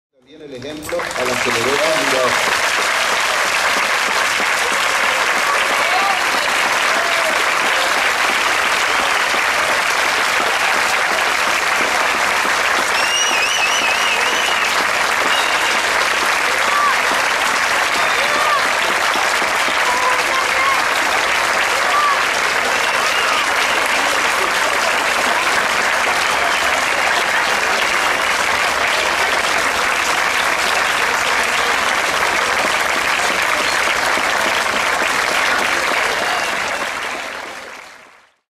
Adeje contra la violencia de género
Adeje se une al aplauso por los Derechos de las Mujeres